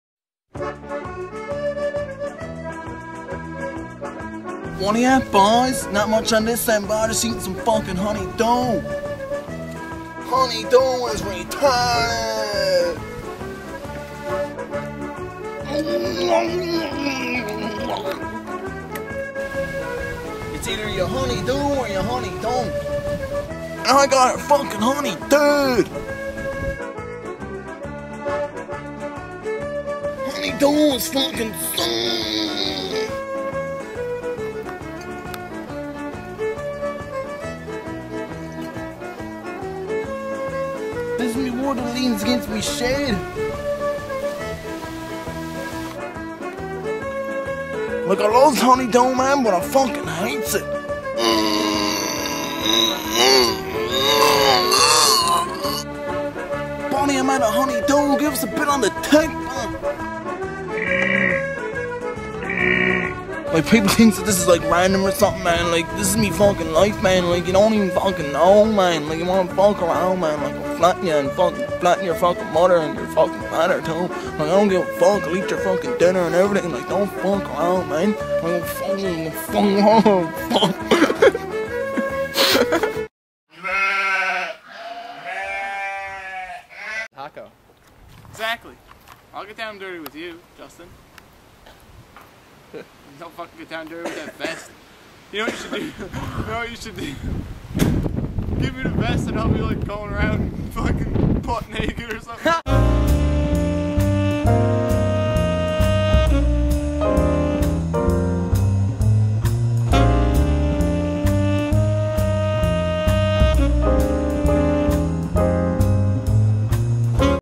Newfoundlander vs Honeydew Melon
This is an accurate visualization of a Newfoundlander eating honeydew melon. If you like Newfoundland and Honeydew Melon This this movie IS FOR YOU
. newfoundland is SIIIIIIIIIIIICK!!!